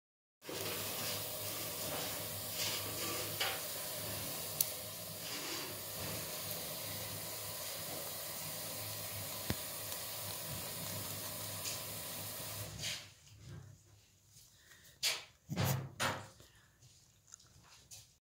Mellerudskaninen Kryddhjärta gnager på en pinne
The Mellerud rabbit Kryddhjärta is eating on a stick. 26 March 2020.